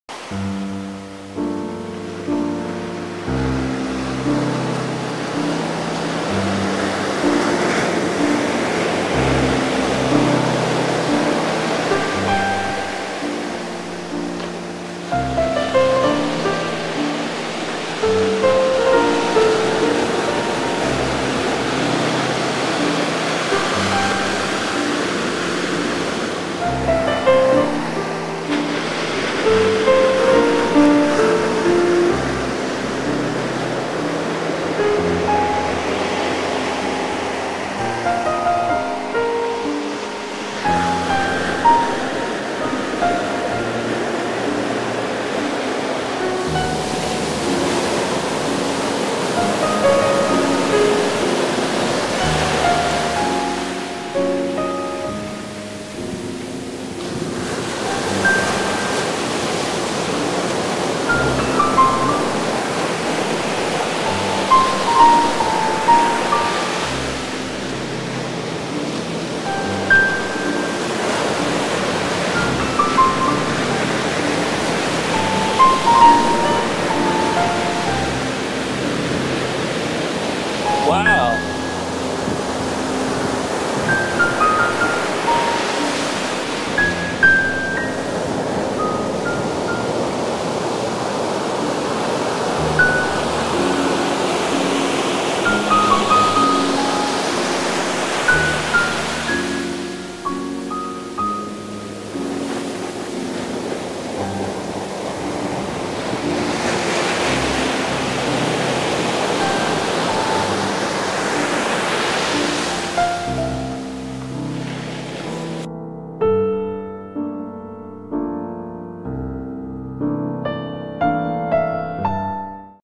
Kimotsuki Video Letter: On the beach in Japan's south
Late afternoon, me and my friend went to Kashiwabara beach, which is about 10-minute drive away from home, to see the effects of the big earthquake in Chile. Fortunately, or unfortunately for that matter, much-feared tsunami waves were not there. Still the waves were higher than usual and we enjoyed feeling the rhythm of the earth and universe.